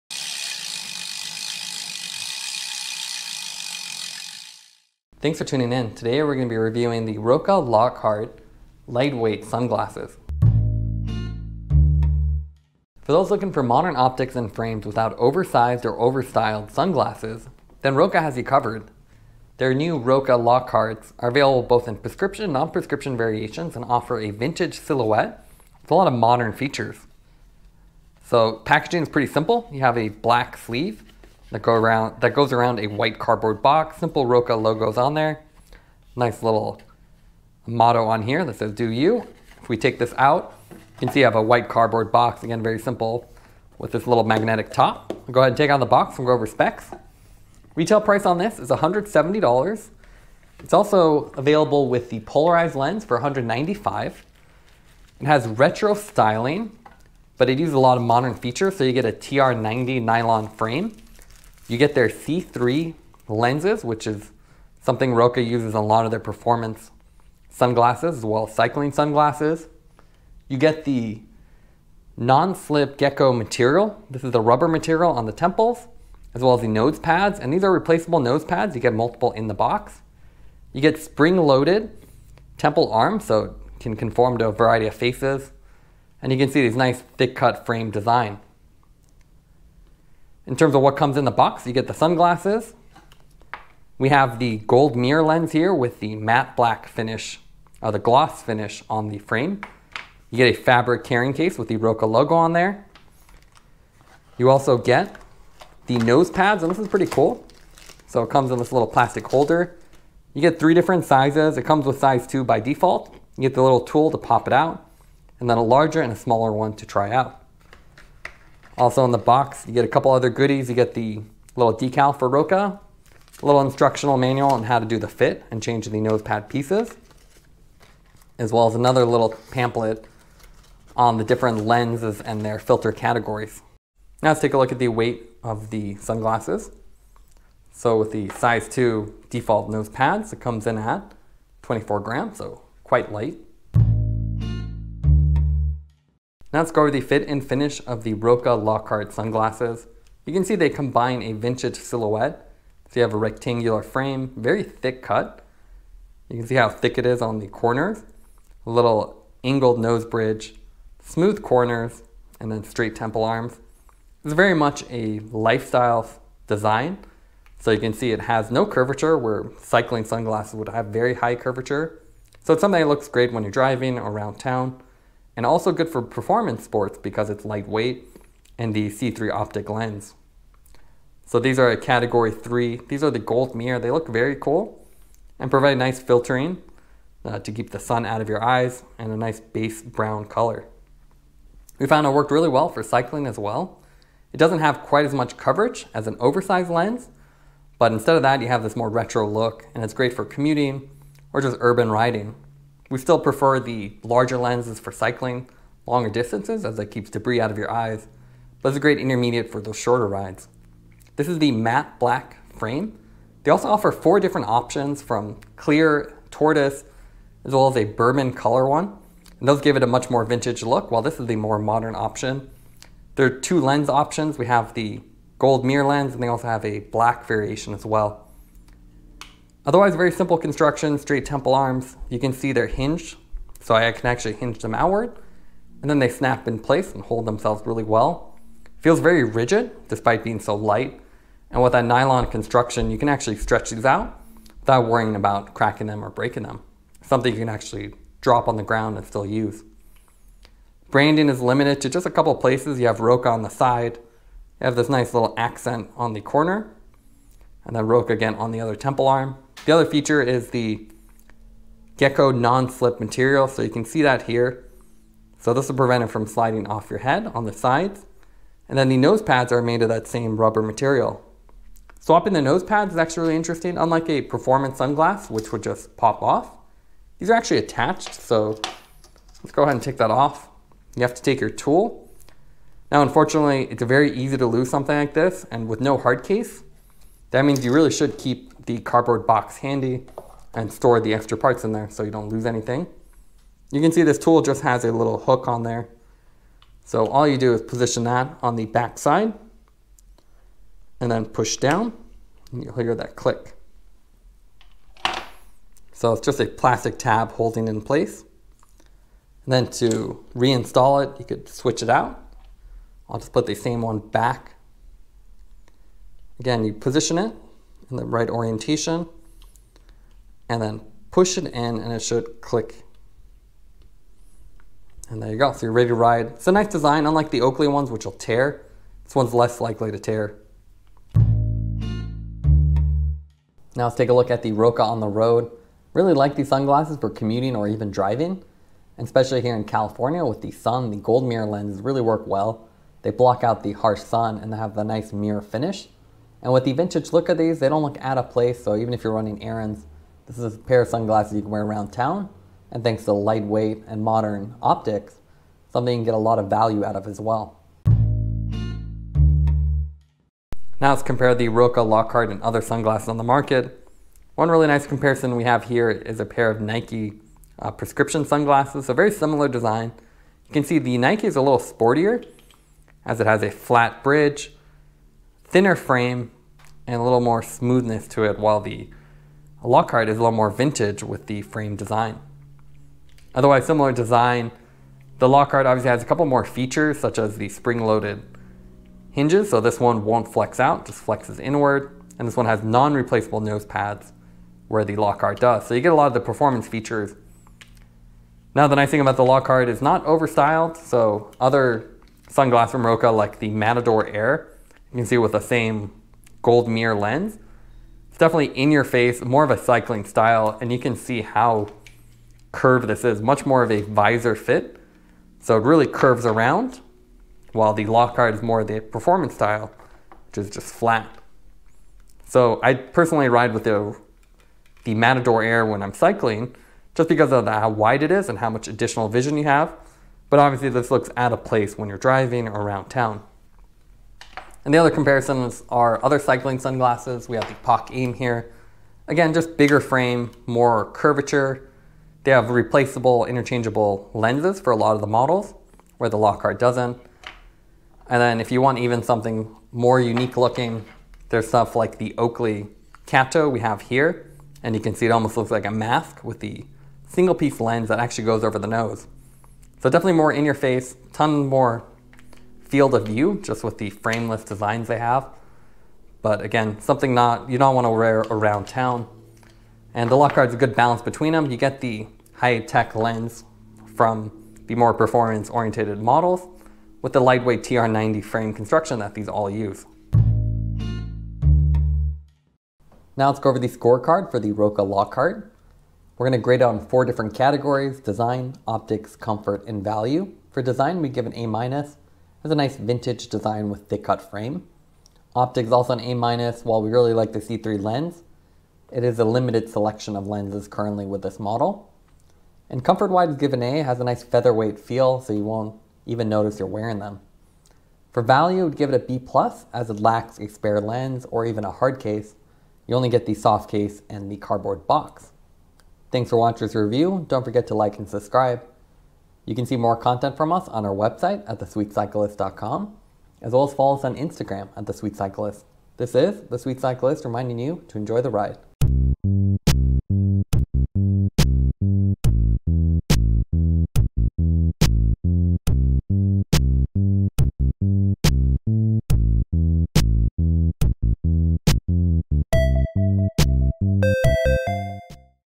ROKA Lockhart Retro Inspired Performance Sunglasses - feat. Prescription Option + Multiple Nosepads
For those looking for modern optics and frames without oversized or overstyled designs, ROKA has you covered with their new Lockhart sunglasses. Available in both prescription and non-prescription options, the Lockhart sunglasses offer a vintage silhouette with modern materials and lenses. Despite the thick cut frames, the Lockhart offers lightweight performance with TR-90 nylon construction and ROKA’s C3 lens options. Retail price varies from $170 for standard lenses and $195 for polarized options. ROKA offers multiple frame colors from modern gloss Black to transparent tortious, White, and Bourbon colors. As with other ROKA sunglasses, the Lockhart features no-slip GEKO material on the temples and nose pads for a comfortable fit.

Unlike other performance running or cycling sunglasses, the Lockhart’s have a restrained retro styling. ROKA describes the Lockhart sunglasses as future-forward and retro-inspired. Marketing jargon aside, the sunglasses have a vintage silhouette with rectangular profile and thick cut frames. ROKA offers multiple frame colors from modern gloss black to more retro transparent tortious, White, and Bourbon colors. In the gloss Black and Gold Mirror Lens combination we chose, the Lockharts have a unique combination of vintage and modern elements. The thick frame may look heavy but because it uses the TR-90 nylon construction, it weighs only 24 grams which is the same as the semi-frameless ROKA Matador Air sunglasses.

The Lockharts are available with ROKA’s premium C3 lens in both prescription and non-prescription options. The C3 lens’s offer distortion free optics and various coatings applied to them to keep them scratch and dirt free. With the lifestyle design of the sunglasses, the lenses themselves are nearly flat. We chose the Gold Mirror lens because they not only look cool but also offer CAT 3 filtering with 16% VLT which works well for sunny weather. The dark bronze base tint on the lenses also helps increase contrast and enhances green and yellow colors. As with other premium lenses from POC or Bollé, the Matador Air also has clear and distortion-free optics. We found the sunglasses worked well for commuting, running, or even going around town...

00:00 Intro
00:10 Unboxing + Specs
02:24 Fit + Finish
05:32 On The Road
05:58 Comparison (ROKA Matador, Nike, Oakley Kato, POC AIM)
08:01 The Final Score